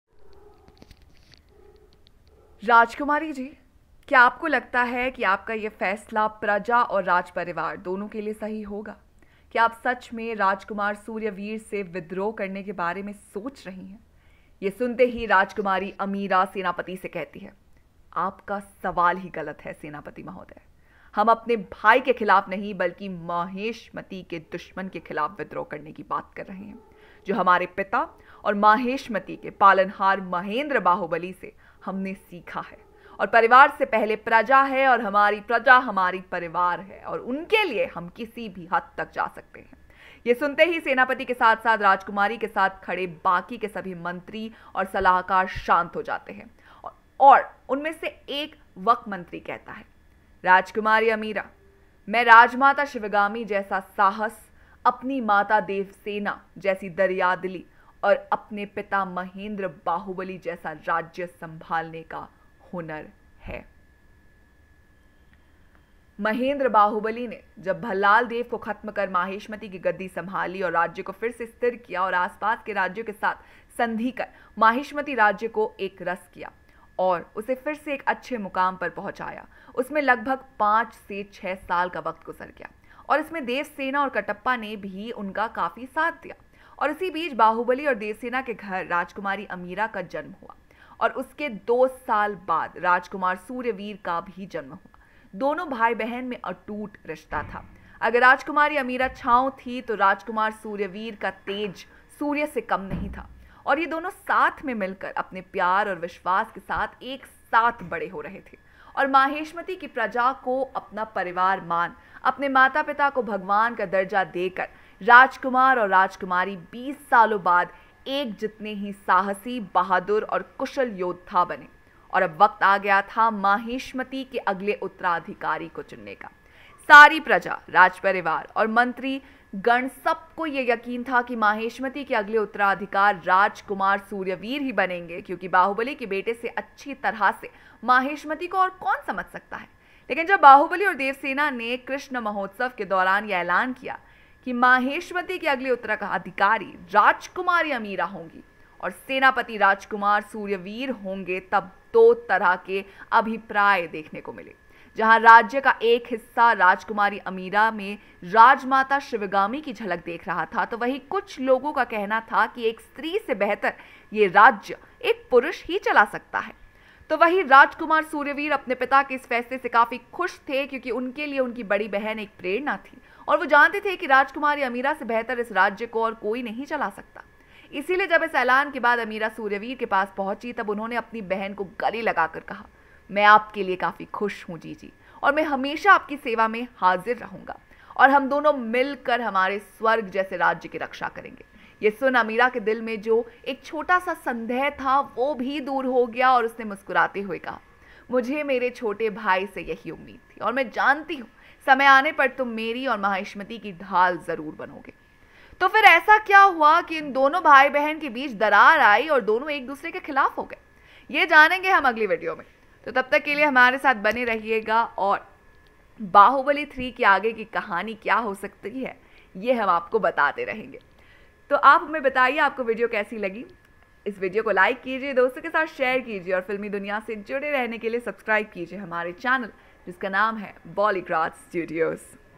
कौन है rajkumari amira? | Bahubali 3 | Anchor - Ispreha | Script - Manisha
Bahubali 3 MV 31/1/2023

Part 1

Thumbnail: कौन है rajkumari amira?

"राजकुमारी जी, क्या आपको लगता है कि आपका ये फैसला प्रजा और राज परिवार दोनों के लिए सही होगा? क्या आप सच में राजकुमार सूर्यवीर से विद्रोह करने के बारे में सोच रही है?'' ये सुनते ही राजकुमारी अमीरा सेनापति से कहती है, ''आपका सवाल ही गलत है सेनापति महोदय, हम अपने भाई के खिलाफ नही बल्कि माहिष्मति के दुश्मन के खिलाफ विद्रोह करने की बात कर रहे है, जो हमारे पिता और माहिष्मति के पालनहार महेंद्र बाहुबली से हमने सीखा है की परिवार से पहले प्रजा है और हमारी प्रजा हमारा परिवार है, और उनके लिए हम किसी भी हद तक जा सकते है।'' ये सुनते ही सेनापति के साथ साथ राजकुमारी के साथ खड़े बाकी के सभी मंत्री और सलाहकार शांत हो जाते है और उन में से वक मंत्री कहते है, ''राजकुमारी अमीरा में राजमाता शिवगामी जैसा साहस, अपनी माता देवसेना जैसी दरियादिली और अपने पिता महेंद्र बाहुबली जैसा राज्य संभालने का हुन्नर है।''

महेंद्र बाहुबली ने जब भलालदेव को ख़त्म कर माहिष्मति की गद्दी संभाली और राज्य को फिर से स्थिर किया और आसपास के राज्यों के साथ संधि कर माहिष्मति राज्य को एकरस किया और उसे फिर से एक अच्छे मुकाम पर पहोंचाया उसमें लगभग पांच से छः साल का वक्त गुज़र गया और इसमें देवसेना और कट्टपा ने भी उनका काफी साथ दिया और इसी बीच बाहुबली और देवसेना के घर राजकुमारी अमीरा का जन्म हुआ और उसके दो साल बाद राजकुमार सूर्यवीर का भी जन्म हुआ, दोनों भाई बहन में अटूट रिश्ता था, अगर राजकुमारी अमीरा छांव थी तो राजकुमार सूर्यवीर का तेज सूर्य से कम नही था और ये दोनों साथ में मिलकर अपने प्यार और विश्वास के साथ एक साथ बड़े हो रहे थे और माहिष्मती की प्रजा को अपना परिवार मान, अपने माता पिता को भगवान का दर्जा दे कर राजकुमार और राजकुमारी 20 सालों बाद एक जितने ही साहसी, बहादुर और कुशल योद्धा बने, और अब वक्त आ गया था माहिष्मती के अगले उत्तराधिकारी को चुनने का।

सारी प्रजा, राजपरिवार और मंत्री गण सब को ये यकीन था कि माहिष्मती के अगले उत्तराधिकारी राजकुमार सूर्यवीर ही बनेंगे क्योंकि बाहुबली के बेटे से अच्छी तरह से माहिष्मती को और कौन समज़ सकता है लेकिन जब बाहुबली और देवसेना ने कृष्ण महोत्सव के दौरान ये ऐलान किया कि माहिष्मती की अगली उत्तराधिकारी राजकुमारी अमीरा होंगी और सेनापति राजकुमार सूर्यवीर होंगे तब दो तरह के अभिप्राय देखने को मिले, जहां राज्य का एक हिस्सा राजकुमारी अमीरा में राजमाता शिवगामी की झलक देख रहा था तो वहीं कुछ लोगों का कहना था कि एक स्त्री से बेहतर ये राज्य एक पुरुष ही चला सकता है। तो वहीं राजकुमार सूर्यवीर अपने पिता के इस फैसले से काफी खुश थे क्योंकि उनके लिए उनकी बड़ी बहन एक प्रेरणा थी और वो जानते थे कि राजकुमारी अमीरा से बेहतर इस राज्य को और कोई चला ही नही सकता। इसलिए जब इस ऐलान के बाद अमीरा सूर्यवीर के पास पहोंची तब उन्होंने अपनी बहन को गले लगा कर कहा, ''मैं आपके लिए काफी खुश ही जीजी, और मैं हमेंशा आपकी सेवा में हाज़िर रहूंगा और हम दोनों मिल हमारे स्वर्ग जैसे राज्य की रक्षा करेंगे'' ये सुन अमीरा के दिल में जो एक छोटा सा संदेह था वो भी दूर हो गया और उसने मुस्कुराते हुए कहा, ''मुझे मेरे छोटे भाई से यही उम्मीद थी और मैं जानती हूं समय आने पर तुम मेरी और माहिष्मती की ढाल ज़रूर बनोगे."

तो फिर ऐसा क्या हुआ कि इन दोनों भाई बहन के बीच दरार आई और दोनों एक दूसरे के खिलाफ हो गए? ये हम जानेंगे next video में, तब तक आप इन video से जुड़े अपने खयाल हमें comments में ज़रूर बताइयेगा।

@manisha vidhani